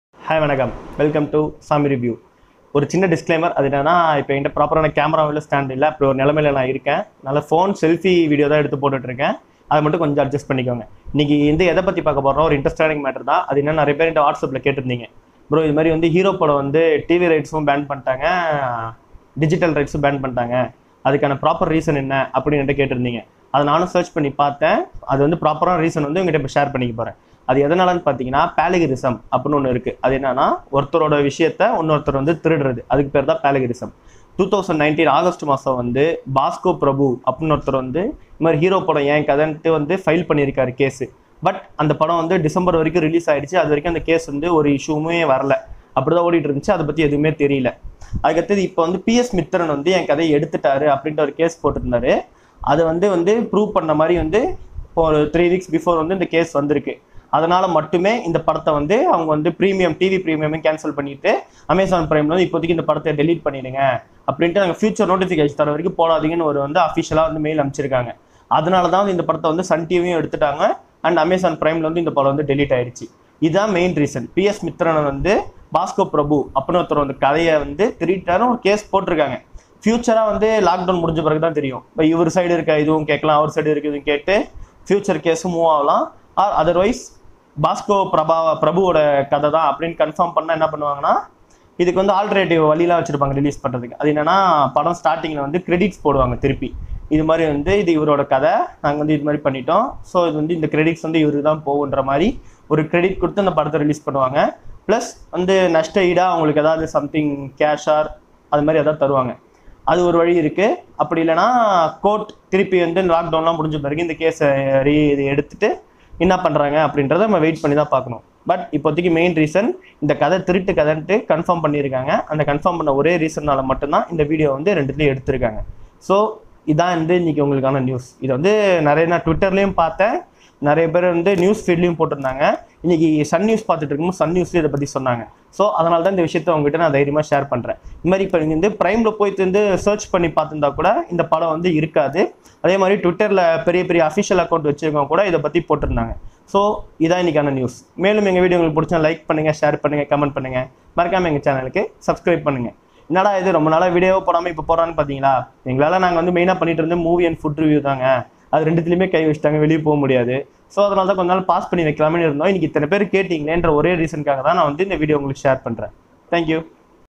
Why HERO movie removed from prime and banned in TV!
Why Hero movie removed from TV premier and Amazon Prime.
Ennadhan aachu Namba Hero vukku ?
Hero 2 Varumaa?